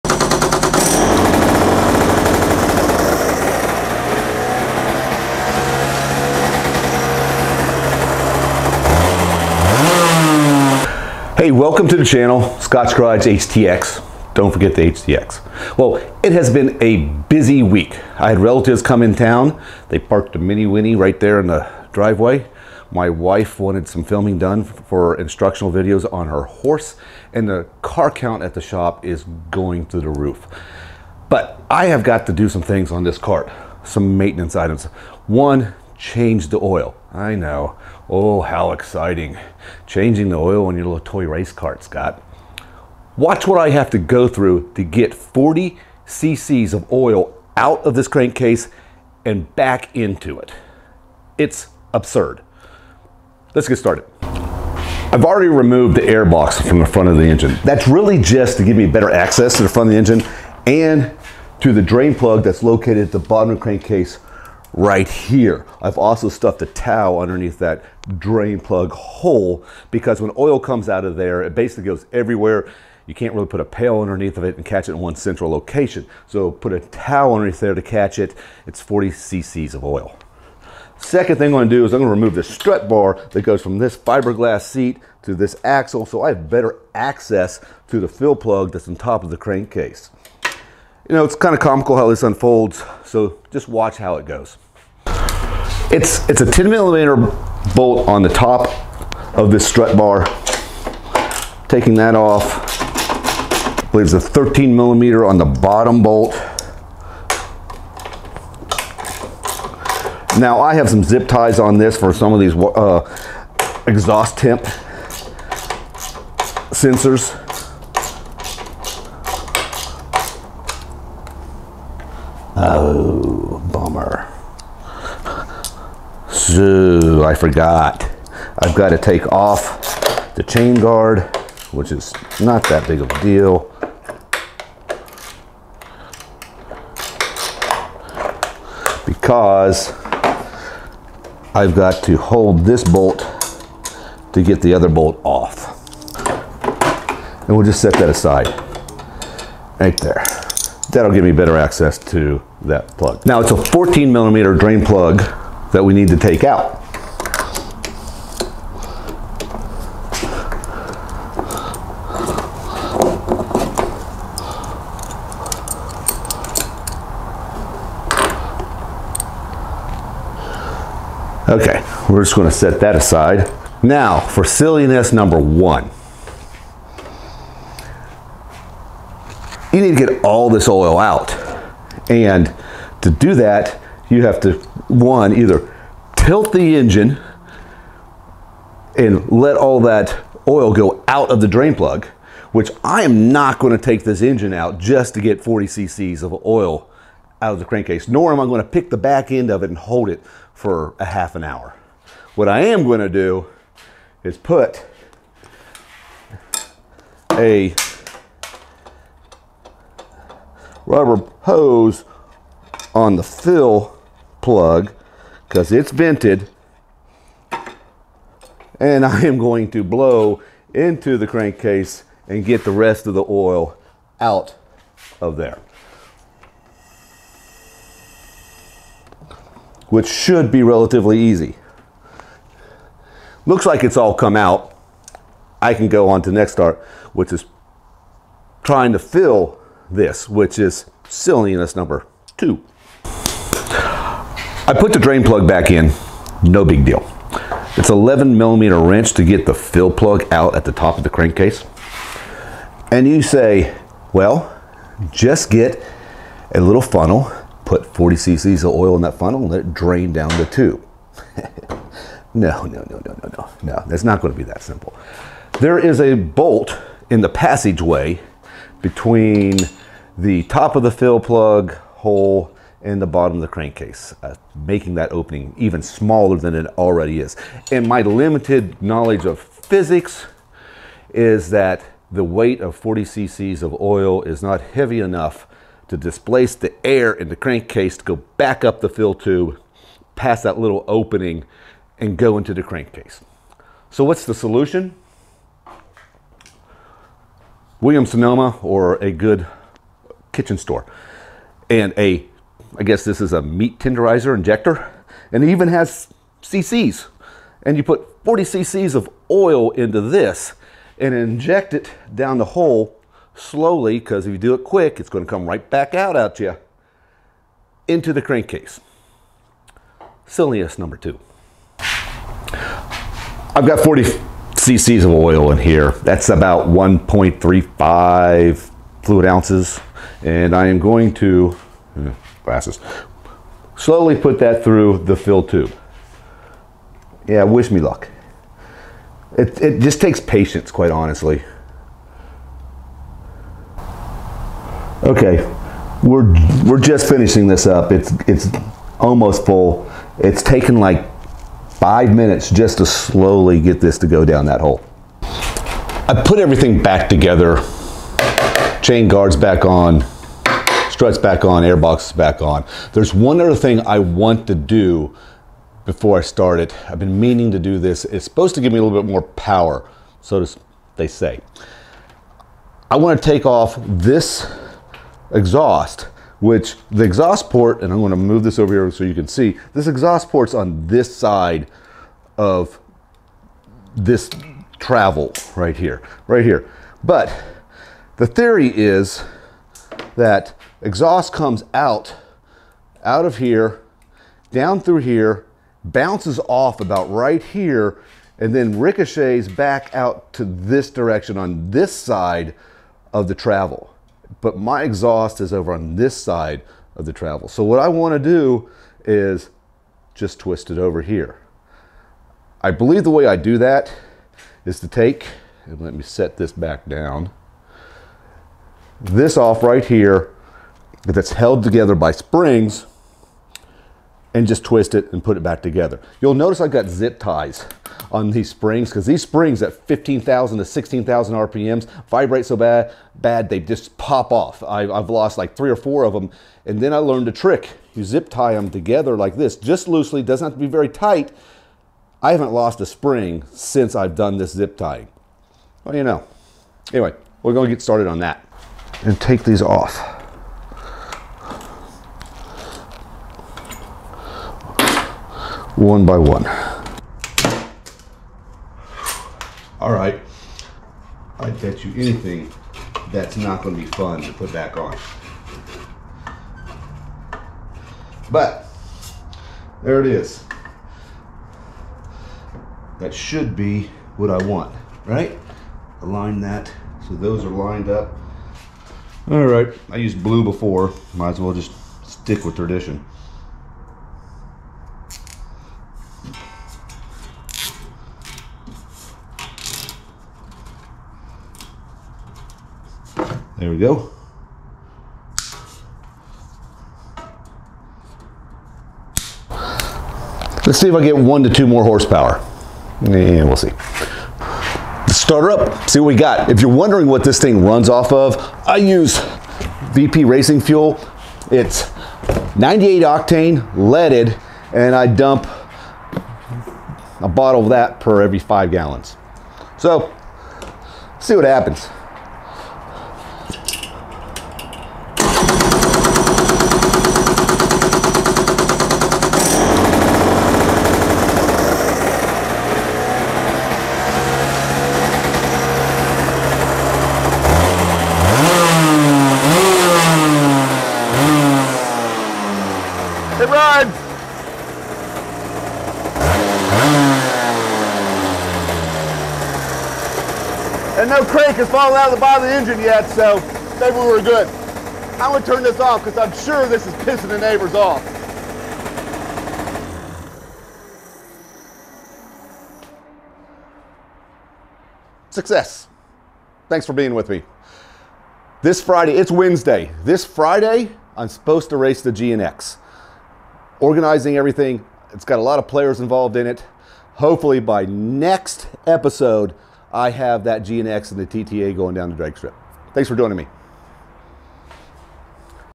The ridiculous process I do to change the oil in the Iame X30 race kart engine
I show you the ridiculous procedure I do to change the oil in the X30 kart engine and a racers tweak to get more horsepower.